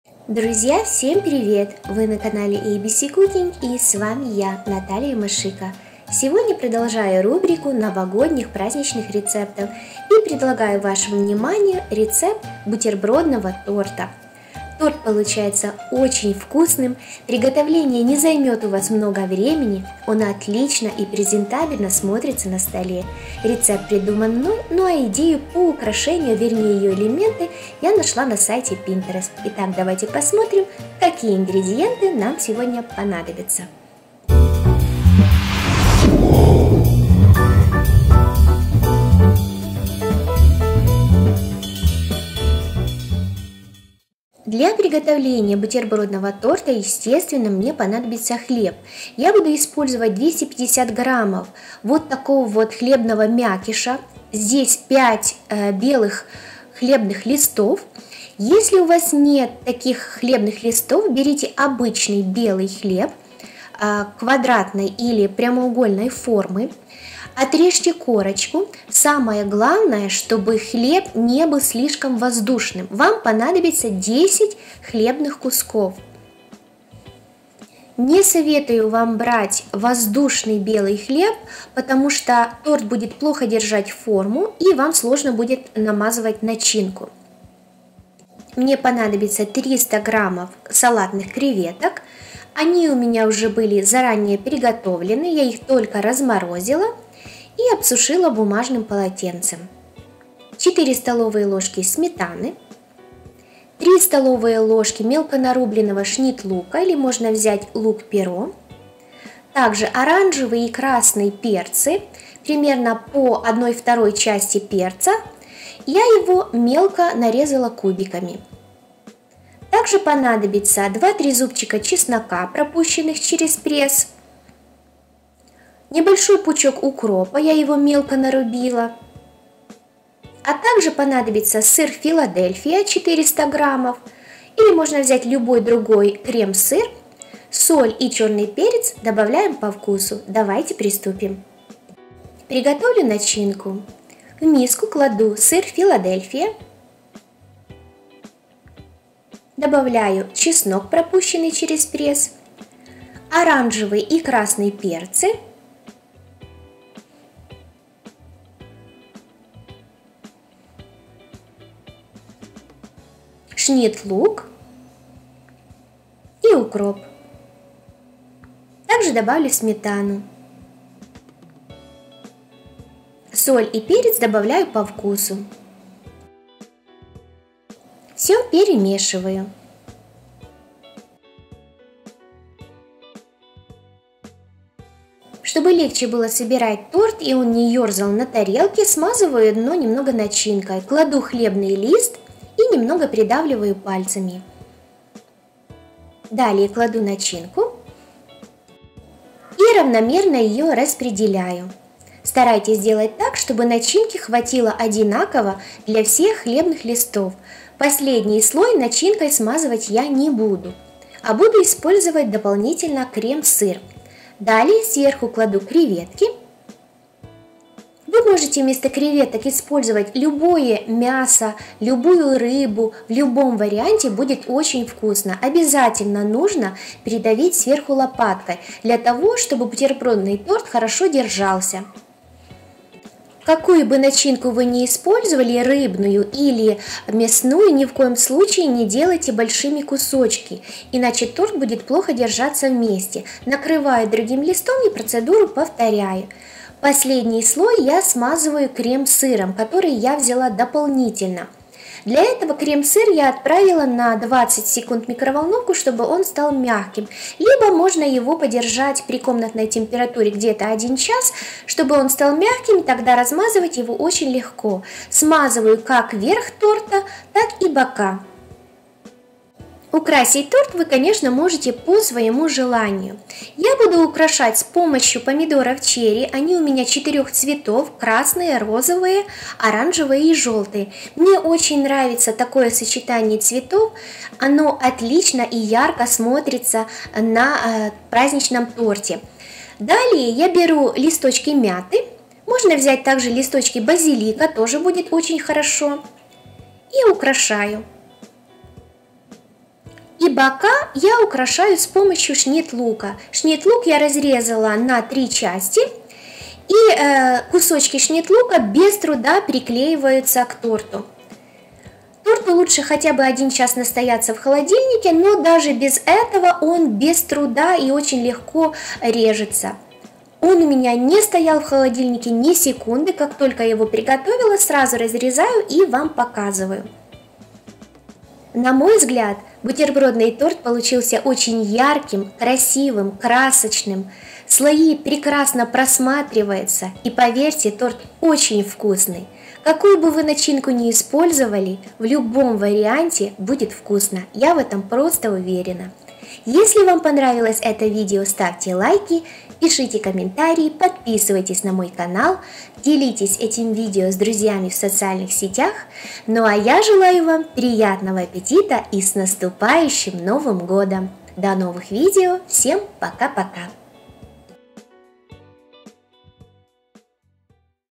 Гости будут в полном восторге от этой закуски! ПРАЗДНИЧНЫЙ БУТЕРБРОДНЫЙ ТОРТ
nataliyamashika Друзья, привет! Закуска получается красочной,яркой, красивой, презентабельной, а самое главное -очень вкусной. Готовится совершенно просто, а результат и вкус действительно впечатляет. Готовьте-не пожалеете! :)
Рецепт:
5 листов(250 г) белого хлебного мякиша( можно заменить 10 кусками белого хлеба без корочки)
300 г салатных креветок
400 г сыра Филадельфия
4 ст.л. сметаны
0,5 красного сладкого перца
0,5 оранжевого сладкого перца
3 ст.л. шнит-лука
2-3 зубчика чеснока
1 небольшой пучок укропа
соль, черный перец по вкусу

Для украшения- 2-3 ст.л. крем сыра+помидоры черри+ шнит-лук+листики мяты

Желаю Вам приятного аппетита и хорошего настроения! :)